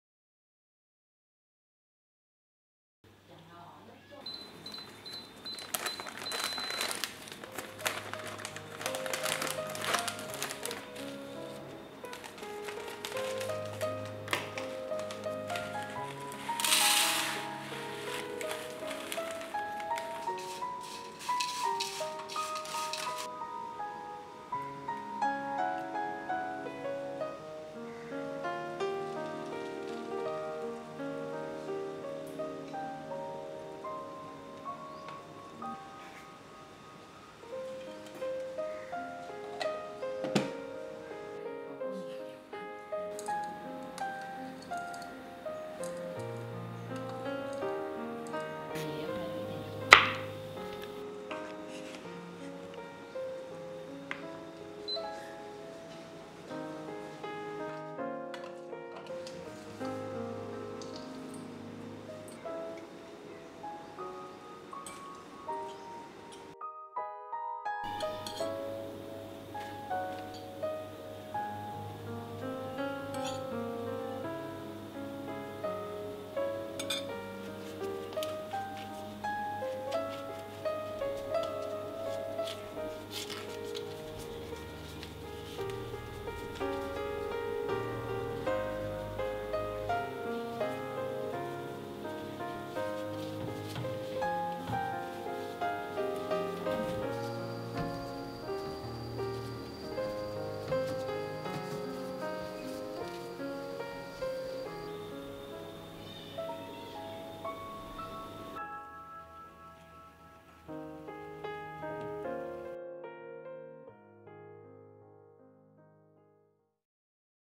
淨斯食譜: 淨斯金黃玉米粽子
淨斯與"粽"不同

淨斯黃金玉米粽子
Vegan Instant Rice with Corn Dumplings

金黃玉米香積飯   60g
調味包                    1包
淨斯大燕麥            20g
生機果仁                40g
沸水                       120g

Vegan Instant Rice with Corn     60g
Seasoning pack                   1 packet
Jing Si Large Oatmeal                 20g
Jing Si Natural Nuts                     40g
 Boiling water                              120g

*以上食材皆可於靜思書軒購買
*The ingredients abie can all be purchased in Jing Si Books & Cafe